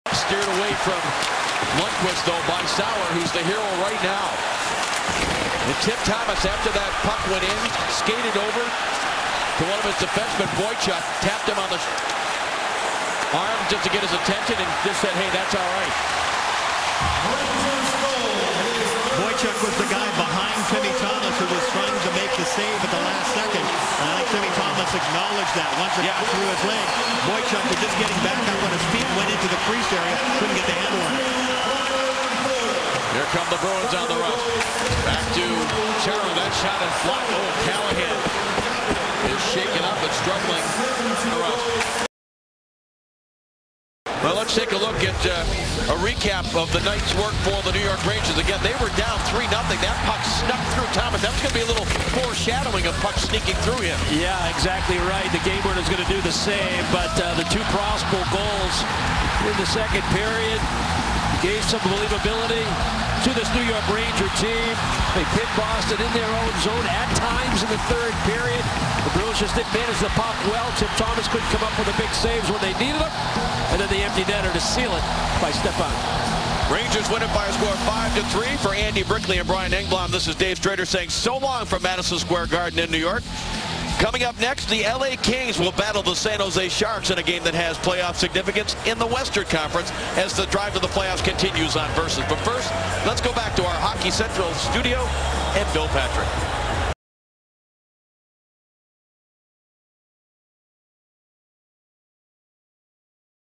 Block from Ryan Callahan 04/04/2011 BOS vs. NYR
Ryan Callahan blocks a blast from Zdeno Chara late in the third period - goes
down for a second and finishes his shift. Callahan has a broken ankle. Out indefinitely long term.
Boston Bruins vs New York Rangers.
Zoom is not the best , but thats because of the camera work on versus.

Short summary of the 5 NYR goals at the end of the video.